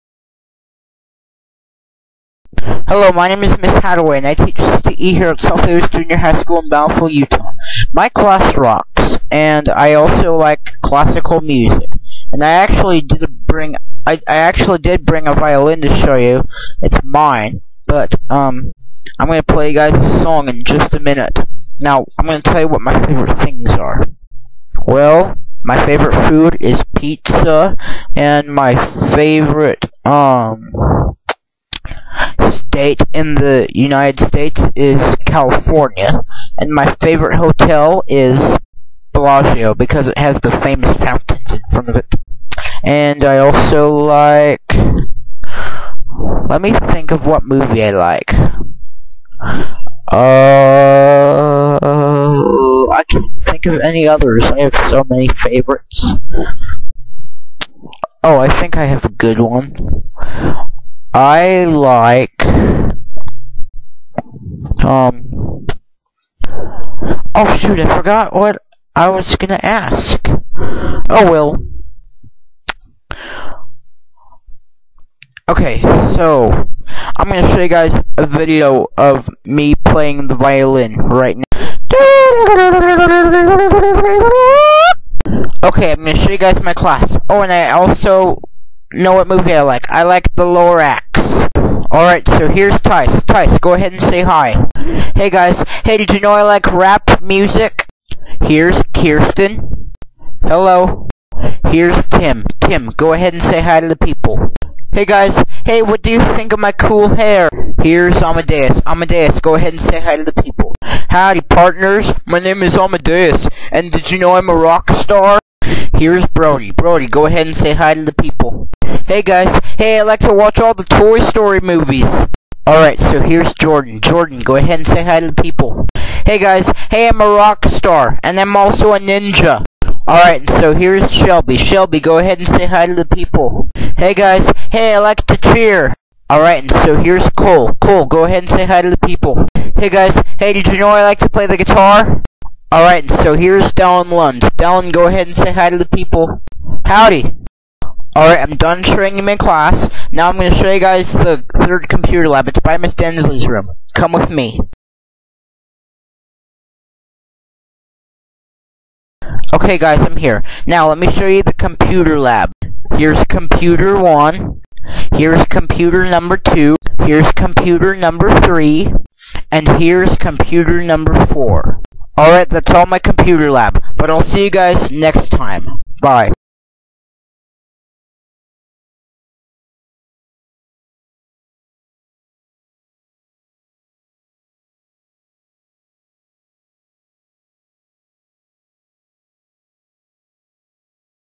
Lego Mrs. Hattaway's CTE Class
This is a video about Mrs. Hattaway teaching her CTE class at South Davis Jr. High School.
Students:
Dallin Lund
Tyce
Kiersten
Brody
Amedeias
Tim
Jordan
Shelby
Cole